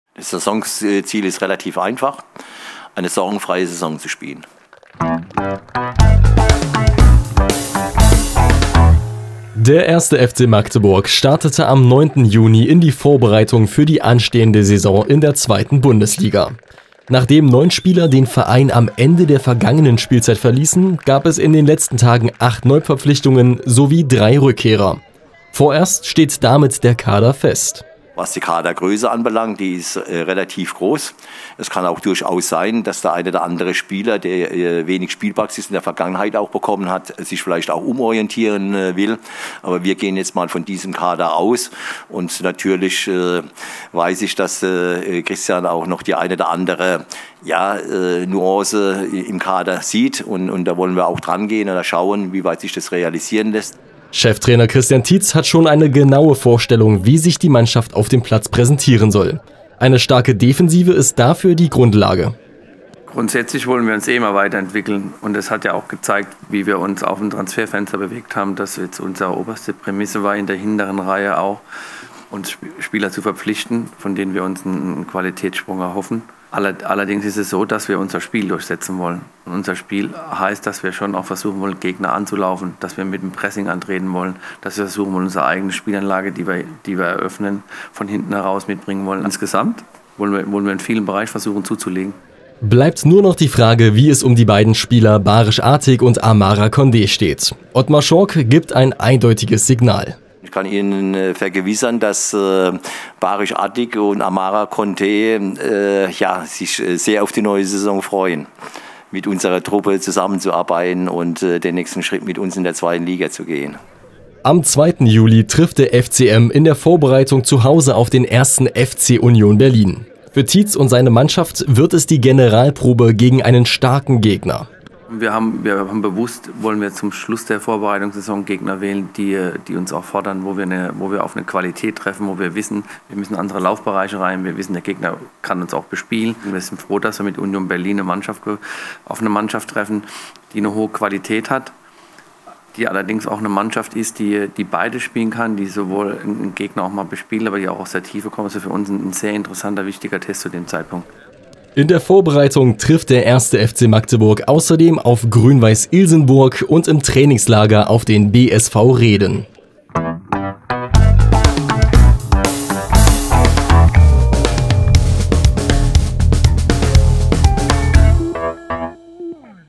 TRAININGSAUFTAKT beim 1. FCM | Magdeburg News
Am 9. Juni startete der 1. FC Magdeburg in die Vorbereitung auf die neue Saison in der 2. Bundesliga. Mit dabei waren auch Baris Atik und Amara Conde sowie zahlreiche Fans des FCM. Cheftrainer Christian Titz und Geschäftsführer Sport Otmar Schork gaben Einblicke in ihre Planungen.